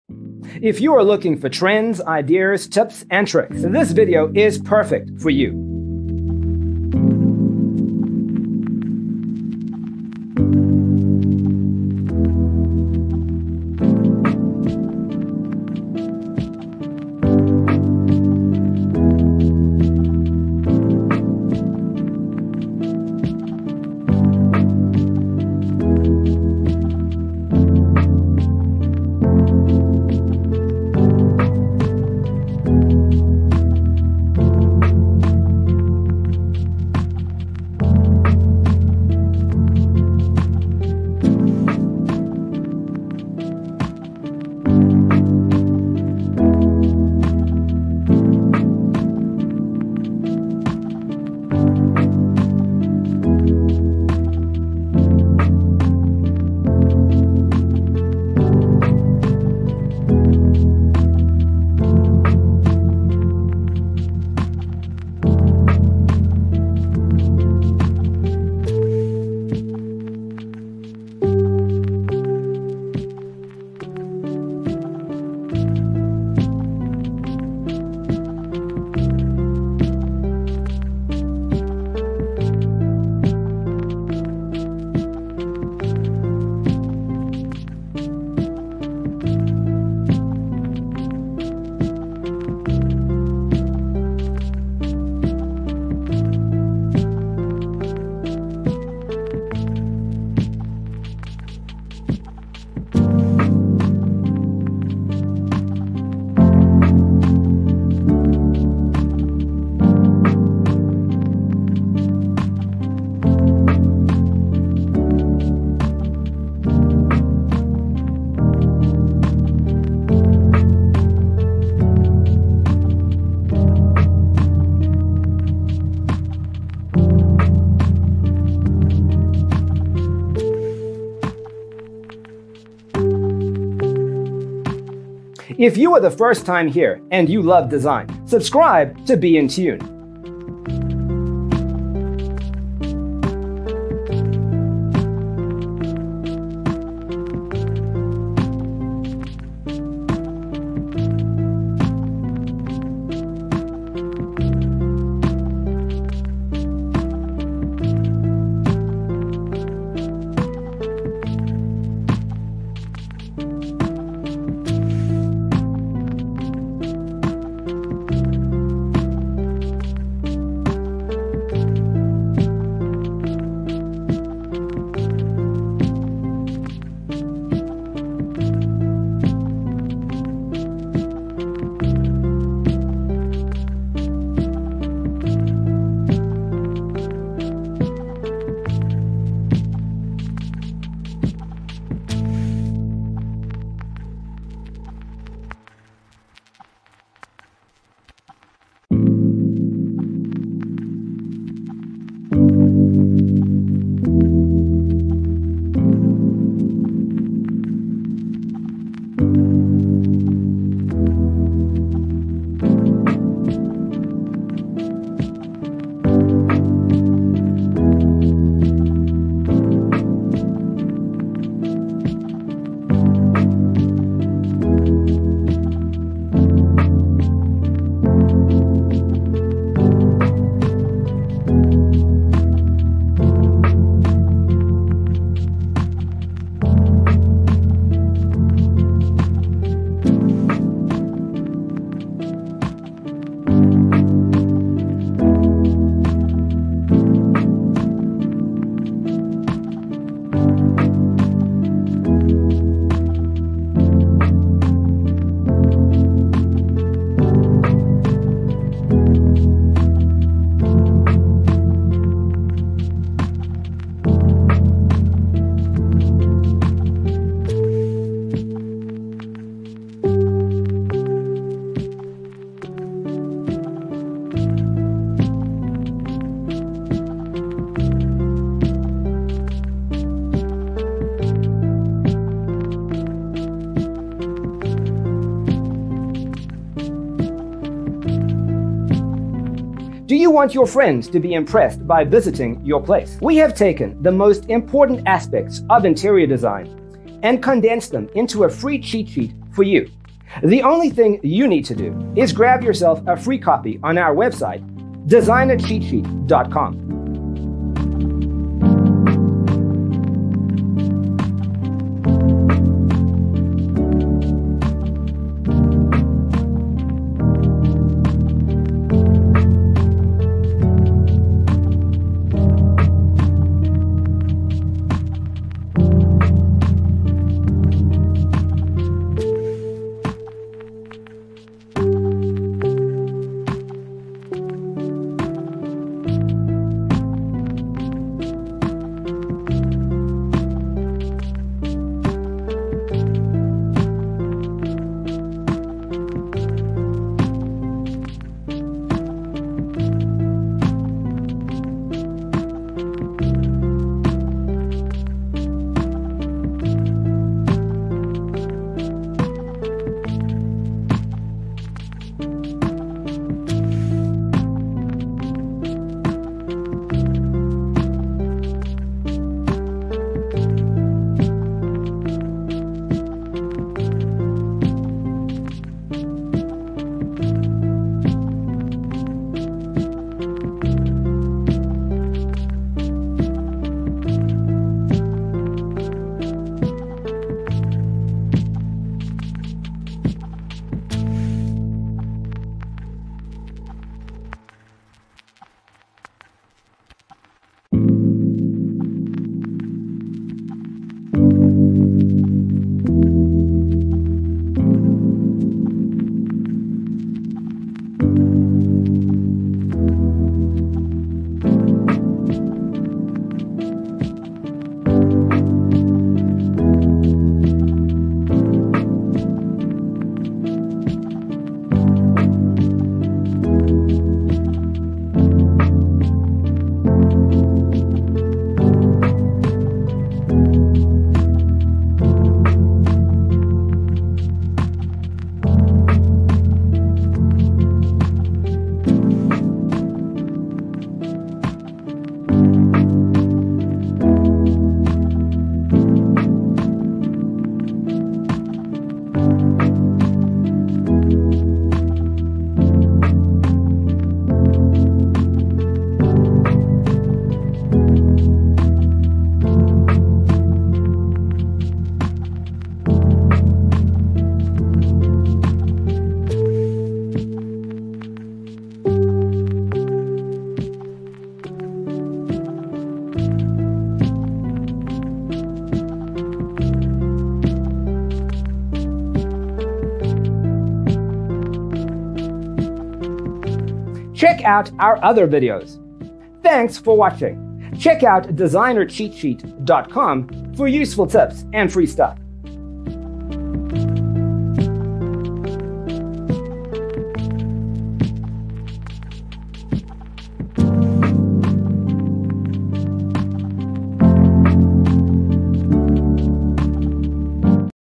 75 All Cabinet Finishes Shabby-chic Style Closet Design Ideas You'll Love 🌈
Browse creative shabby-chic style closets and decor inspiration. Discover a variety of storage and shabby-chic style closet ideas, including layout and organization options.

0:00:00 - Intro
0:00:12 - All Cabinet Finishes Shabby-chic Style Closet Design Ideas You'll Love: Inspirations 1 to 25
0:02:56 - All Cabinet Finishes Shabby-chic Style Closet Design Ideas You'll Love: Inspirations 26 to 50
0:05:53 - All Cabinet Finishes Shabby-chic Style Closet Design Ideas You'll Love: Inspirations 51 to 75

Hello there!🌈